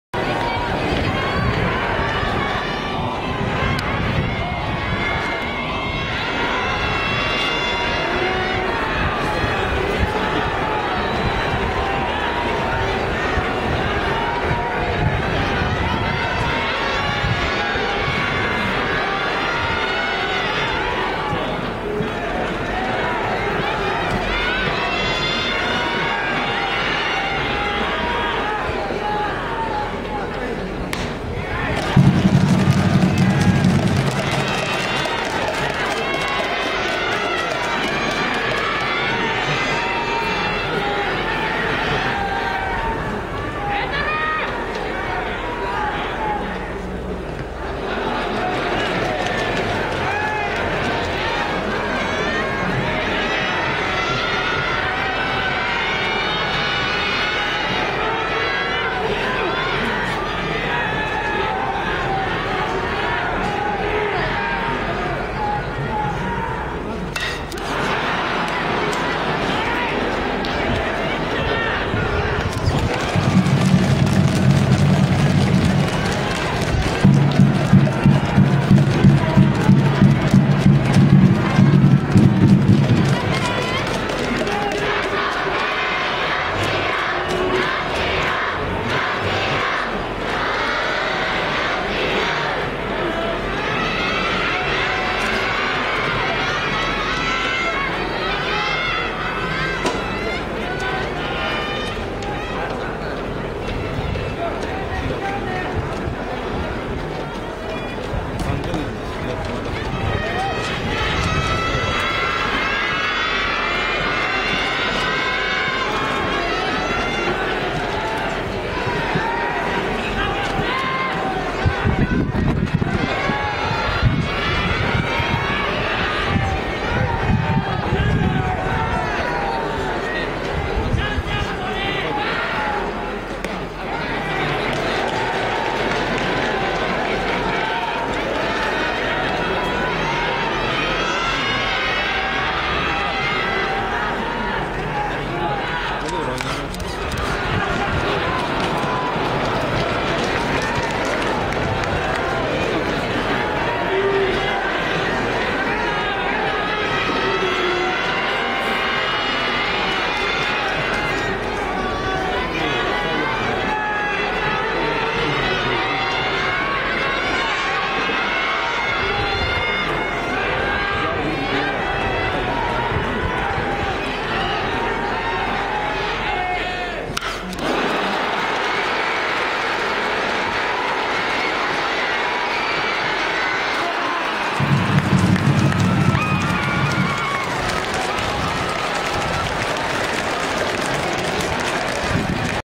野村vs小松島 -2- 13回ウラ
2010年5月1日　春季四国大会１回戦　野村vs小松島　１３回ウラ　野村の攻撃　１－１の同点で迎えた野村のサヨナラ機を小松島守り切る